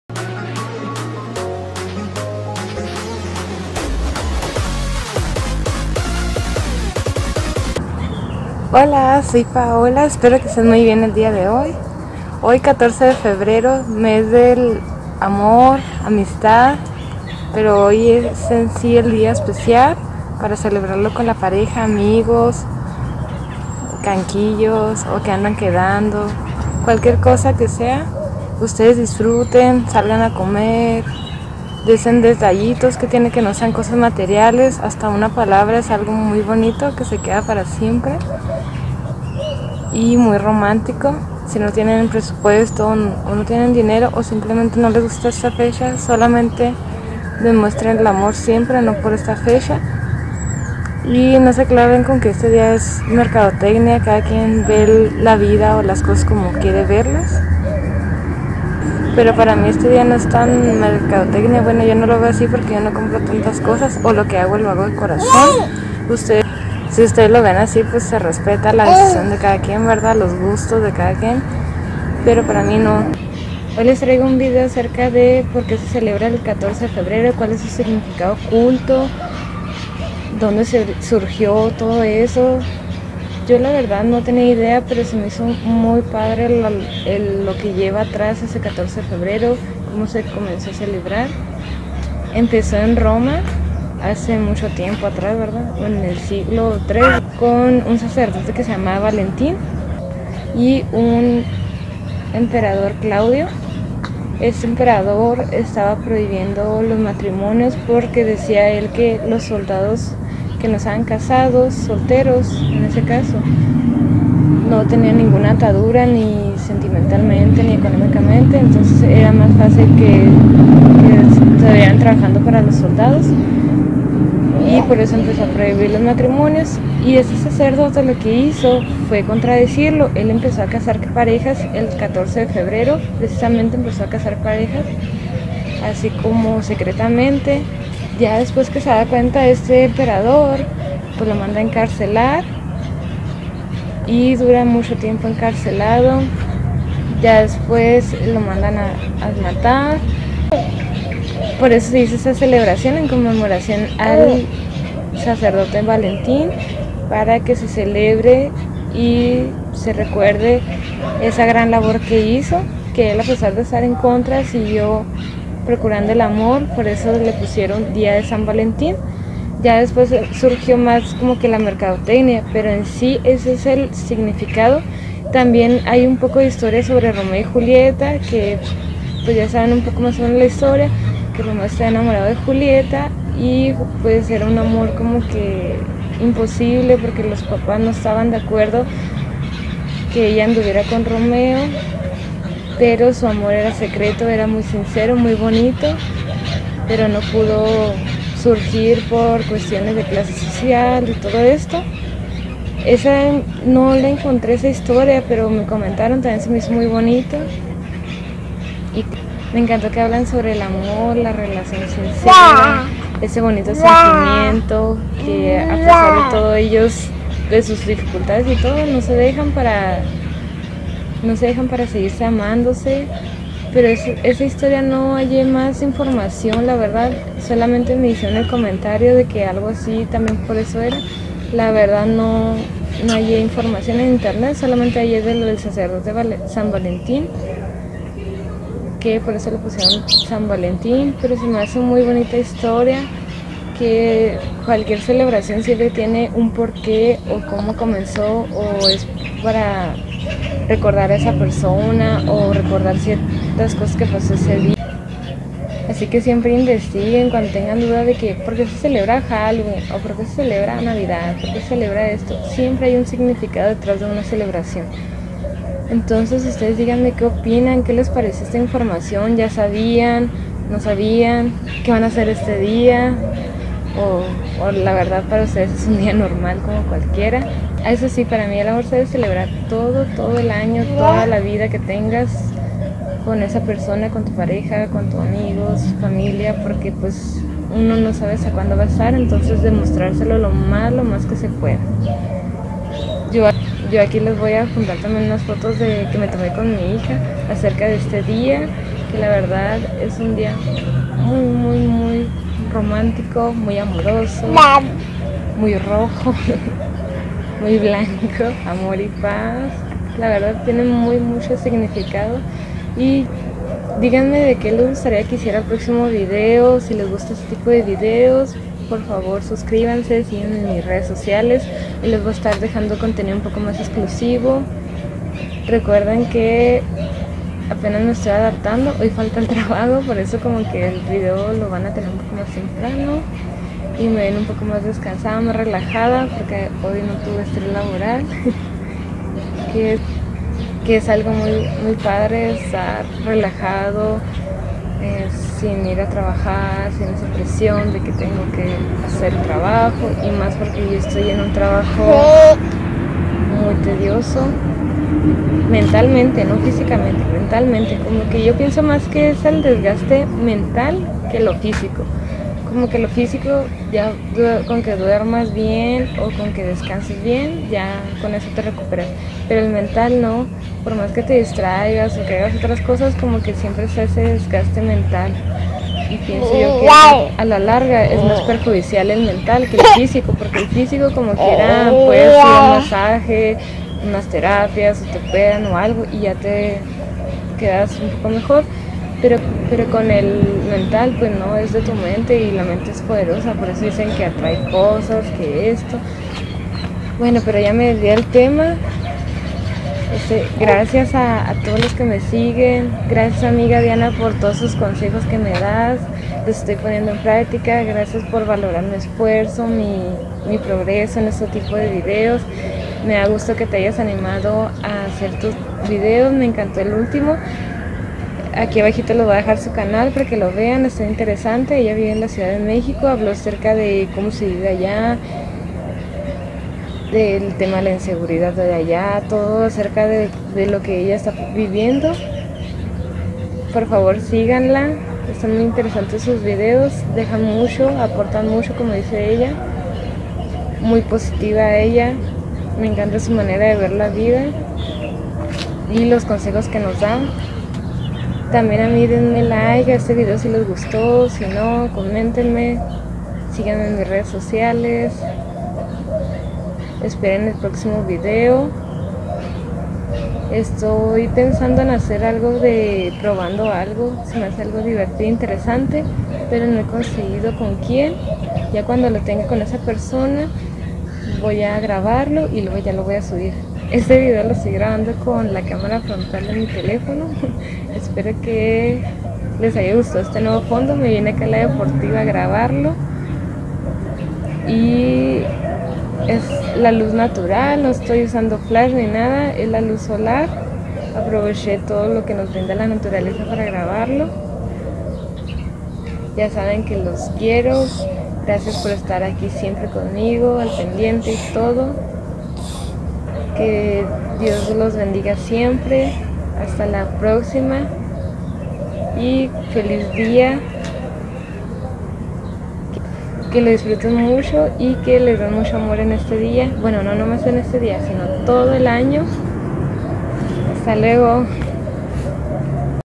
¿PÓRQUE SE CELEBRA EL DÍA DEL AMOR Y LA AMISTAD?
En este vídeo les digo cual es el significado acerca de esta celebración, les dejo unas fotos de este día, unos consejos.

Bendiciones y saludos!

Comenten de que hago mi próximo vídeo.